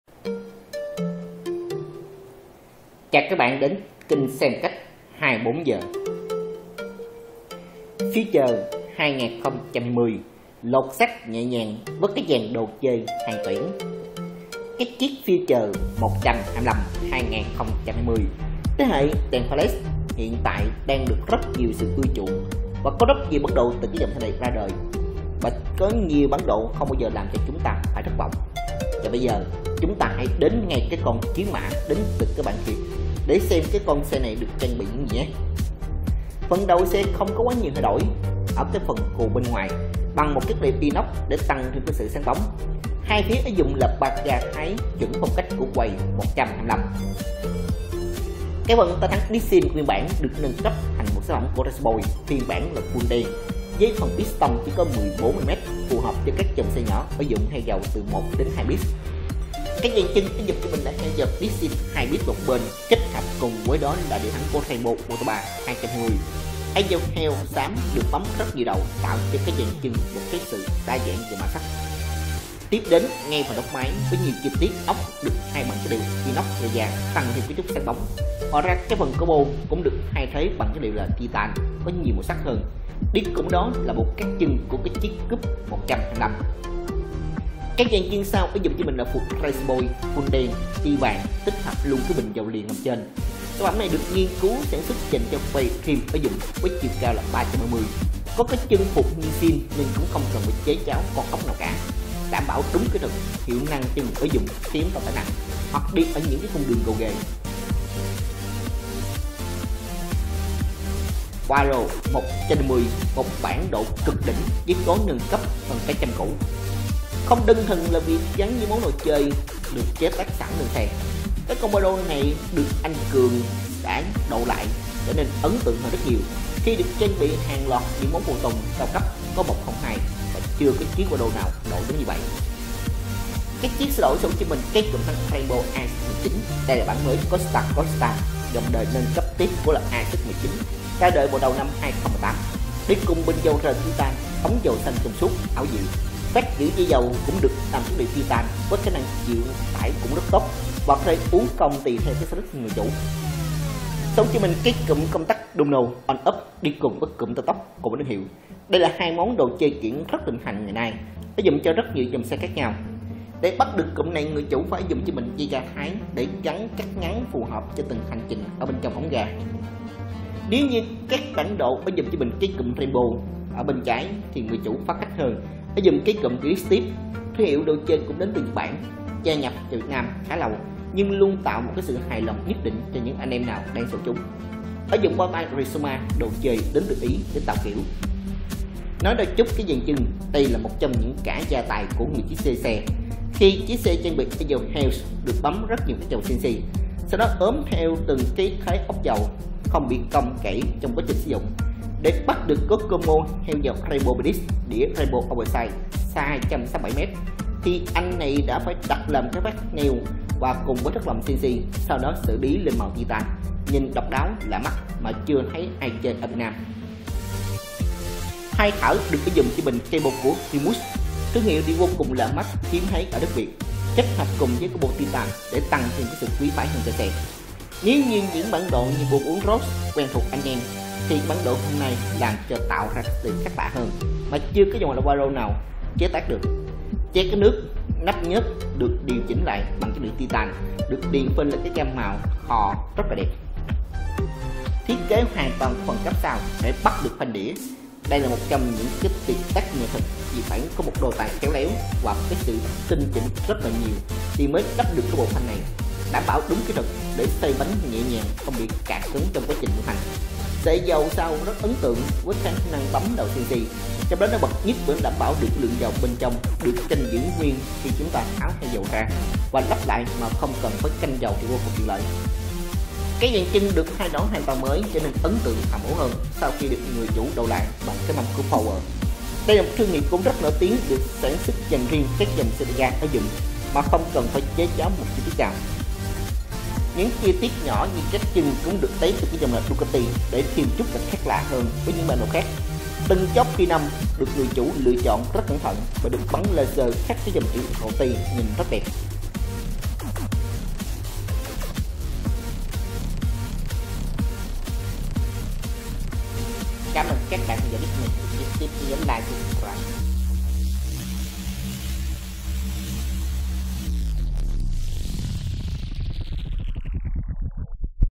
Xe & Phong Cách 24h - Future 2020 nâng cấp nhẹ nhàng với dàn trang bị hàng tuyển.
Future 125 thế hệ đèn led hiện tại đang rất được ưa chuộng và những bản độ từ mẫu xe này chưa bao giờ làm người xem phải thất vọng cả. Bây giờ chúng ta sẽ đến ngay với con chiếc mã của bạn Hiệp, để xem nó được trang bị những gì nhé.